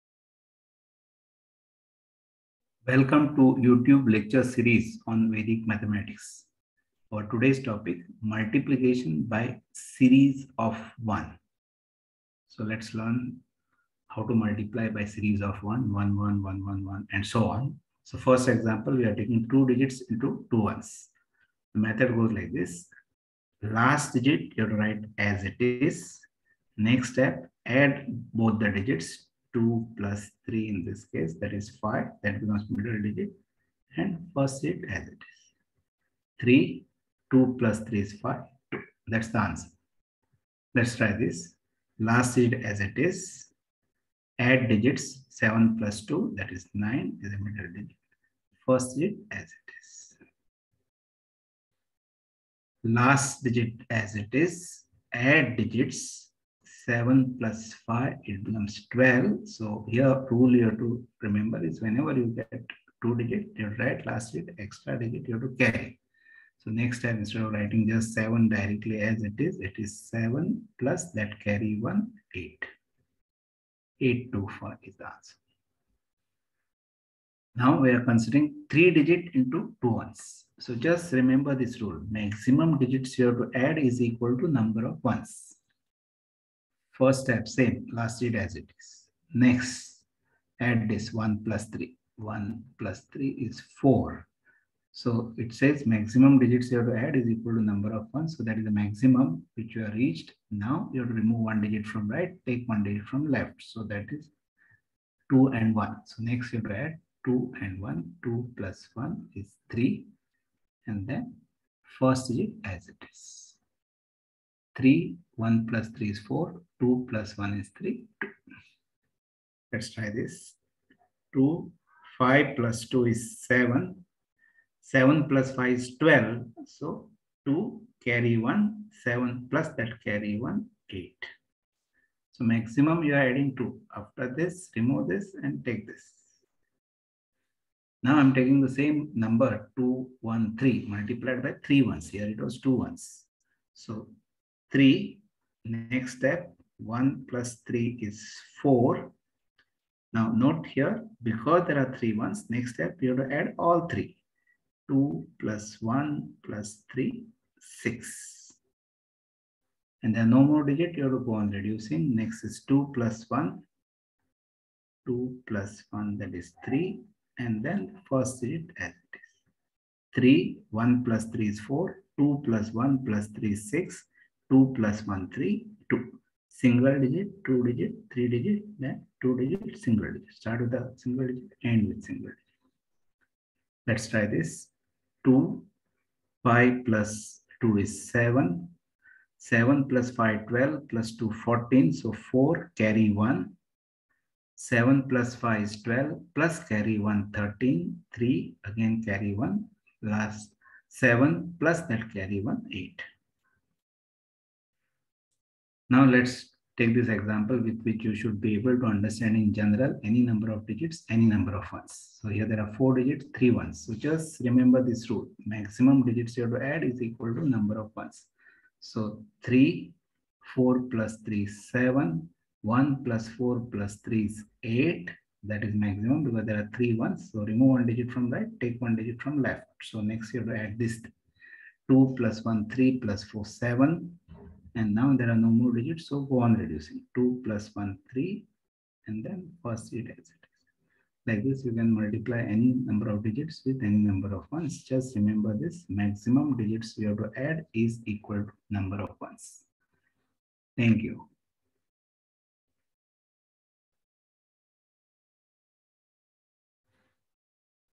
Multiplication by Series of 1
This is lecture series of Vedic Mathematics by SRS Vedic Mathematics Academy. In this session, we are going to learn Multiplication by Series of 1.